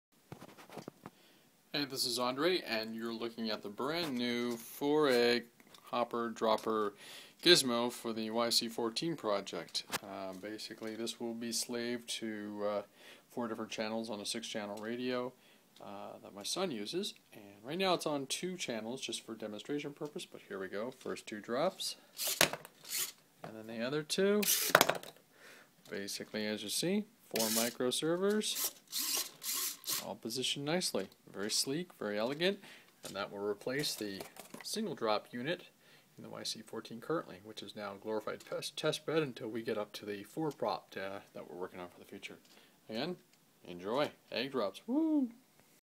YC-14 Four Egg Hopper
New design for the RC plane allowing us to drop four plastic eggs vs one.

Still need to add bay doors under the hopper.